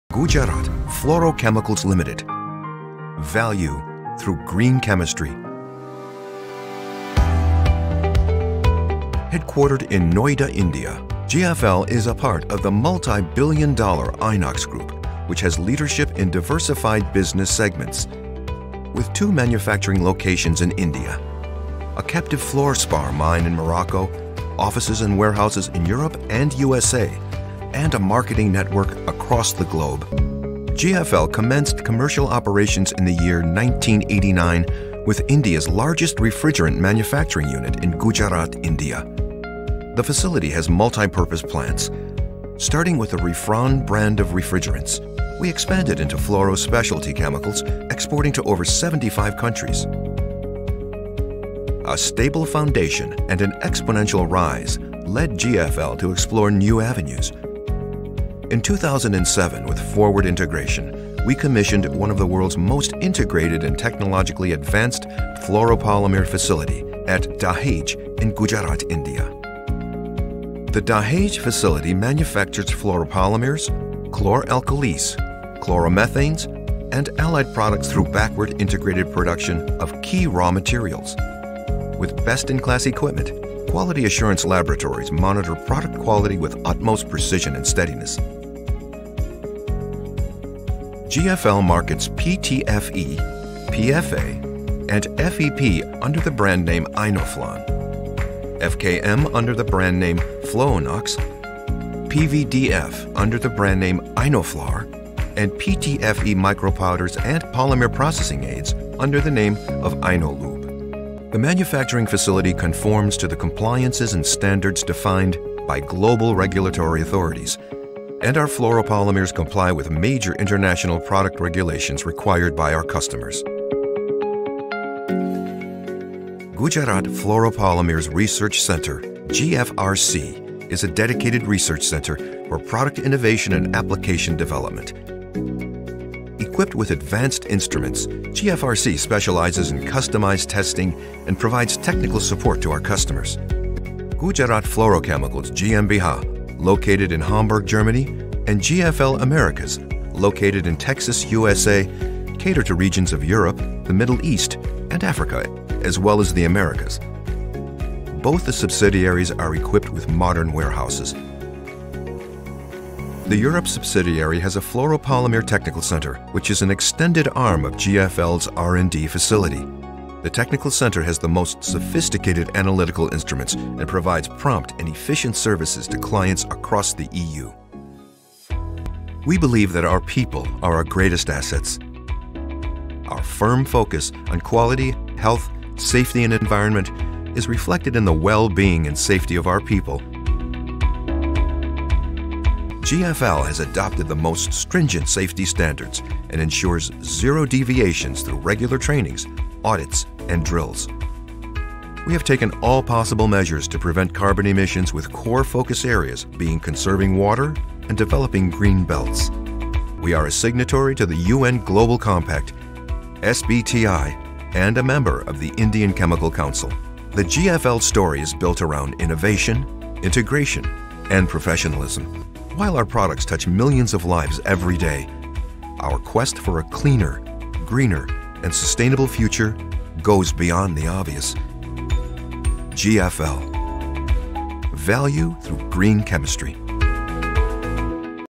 GUJARAT FLUOROCHEMICALS - CORPORATE PROFILE
An ISO 9001:2015, ISO 14001:2015, ISO 45001:2018 and SA 8000:2014 certified organisation, GFL is a leading producer of Fluoropolymers, Fluorospecialities, Refrigerants and Chemicals for applications in varied industries. GFL derives its strength from expertise in Fluorine Chemistry, vertical integration from natural minerals to Fluoropolymers and strong R&D, enabling it to provide one of the best quality products meeting all regulatory compliances, to our clientele globally.